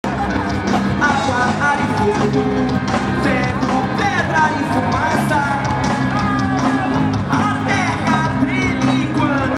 Mega Ramp Sao Paulo 2011 07 02 Mega Rampa 204
Todas as descidas da Mega Rampa em Sao Paulo 2011, BMX e Skate, acompanhem pelo sequencial dos videos. Mega Ramp Sao Paulo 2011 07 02 Mega Rampa